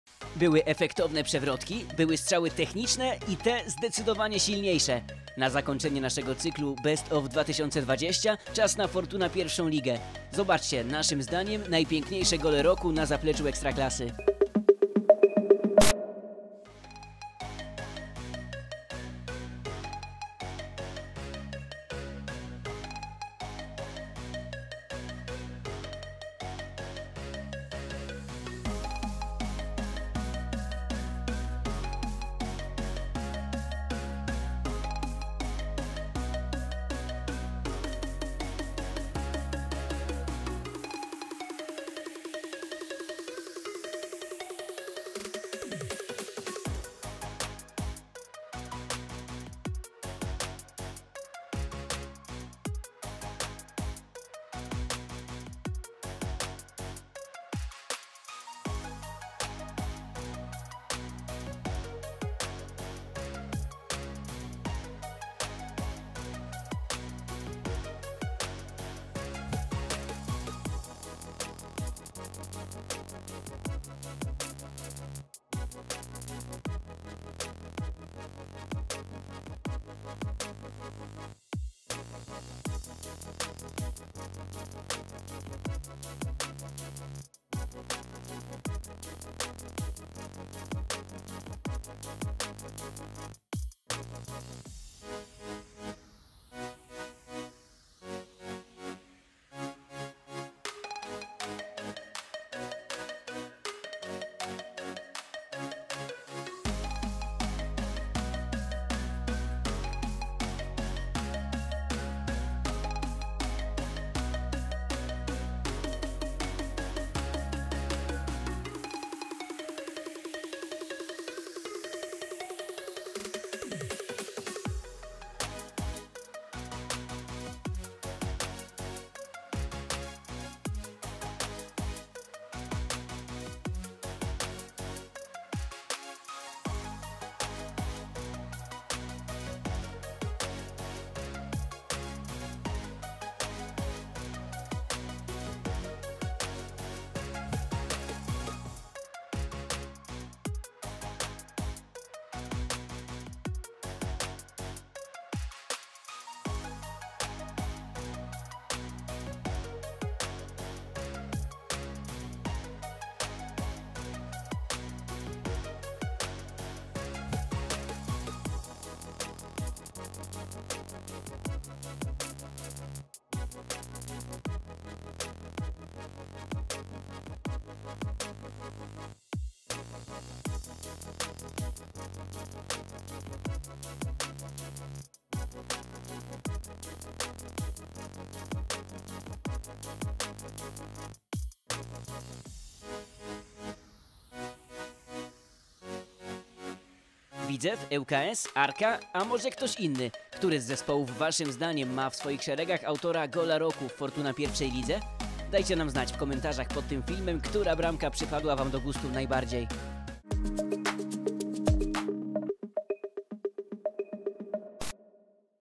BEST of 2020: TOP 10 GOLI z Fortuna 1. Ligi
Na zakończenie naszego cyklu BestOf2020 mamy dla Was najefektowniejsze trafienia z Fortuna 1. Ligi! Sami zobaczcie, jakie gole padły w minionych 12 miesiącach na zapleczu ekstraklasy, a później wskażcie nam tę jedną, najpiękniejszą bramkę! Czekamy na Wasze typy w komentarzach pod tym filmem!

Oficjalny kanał Polskiego Związku Piłki Nożnej. / Official channel of the Polish Football Association.